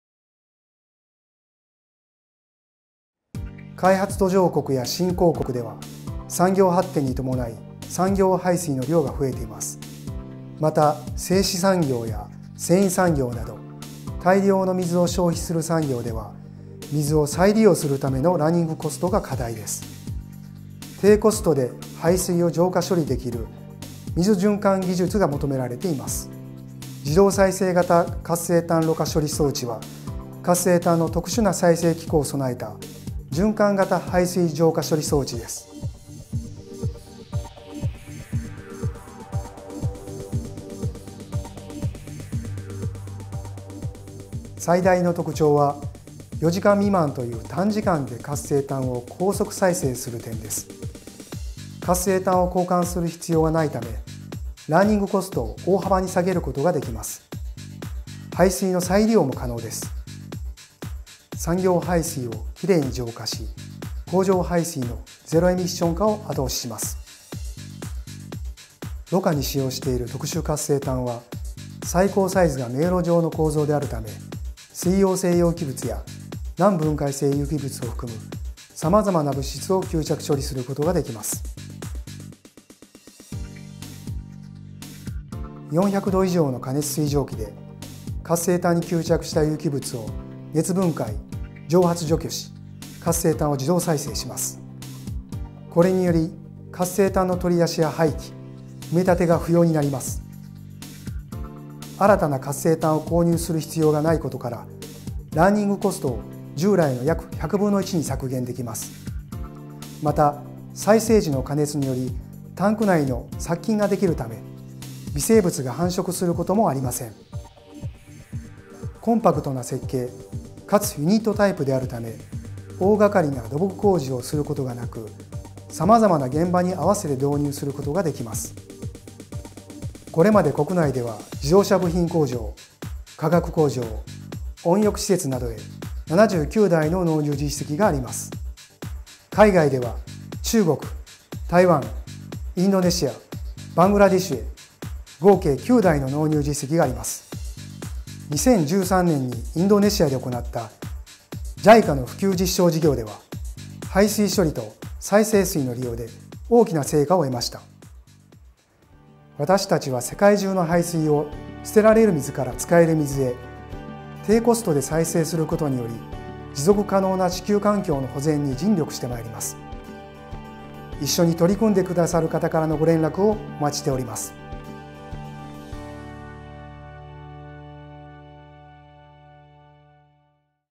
「はばたく300社 2020」Jトップ 株式会社 PR動画
2020年11月12日、ITサービス導入や経営資源の有効活用等による生産性向上、積極的な海外展開やインバウンド需要の取込み、多様な人材活用や円滑な事業承継など、様々な分野で活躍している中小企業・小規模事業者を「はばたく中小企業・小規模事業者300社」として選定し、授賞式を執り行いました。

本動画は選定事業者のPR動画になります。
「はばたく中小企業・小規模事業者300社」2020